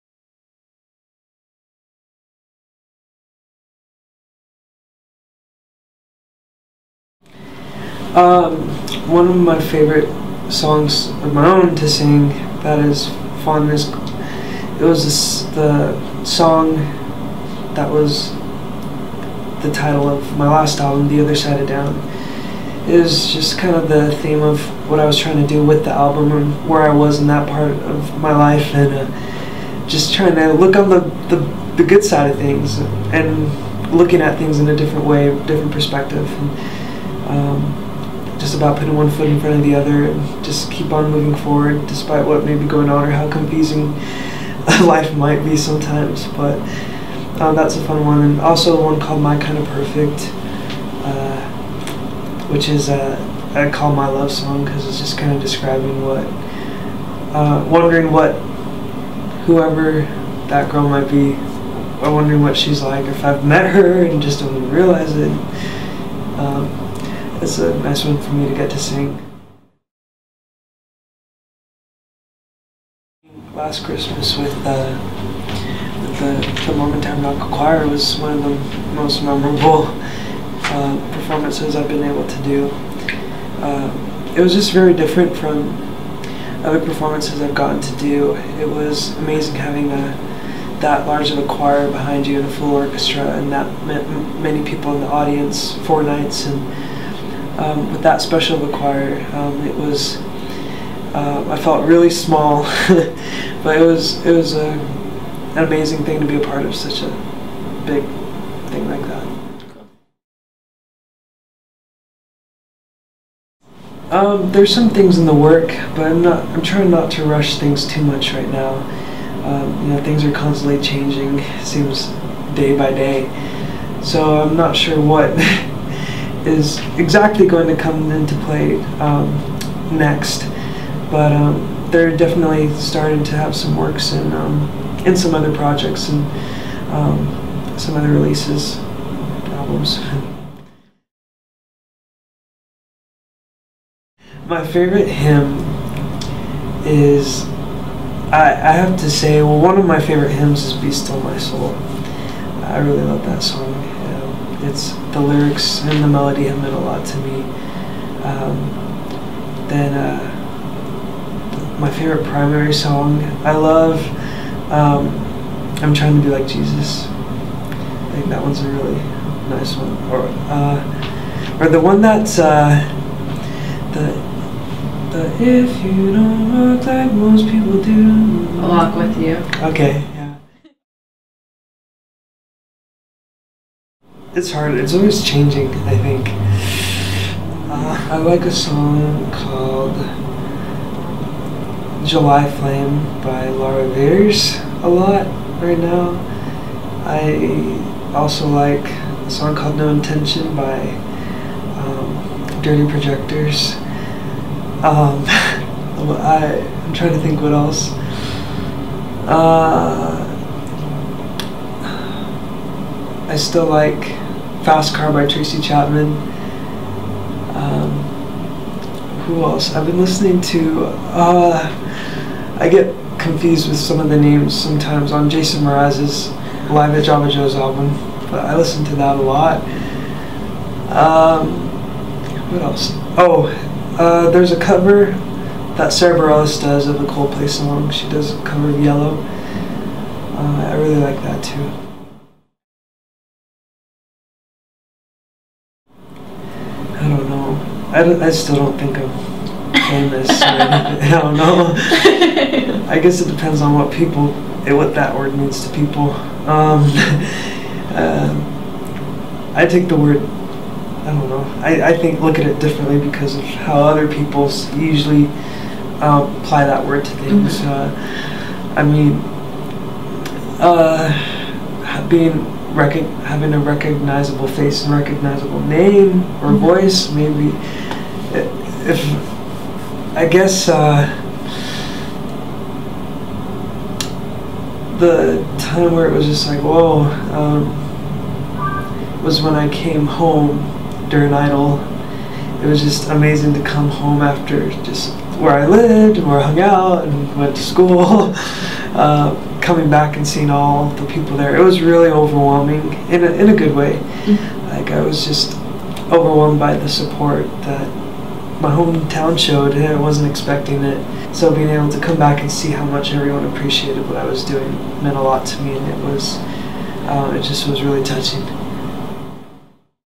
Interview with David Archuleta - Deseret Book
LDS Living invited their readers to submit questions they wanted to hear David Archuleta answer... hear what he has to say!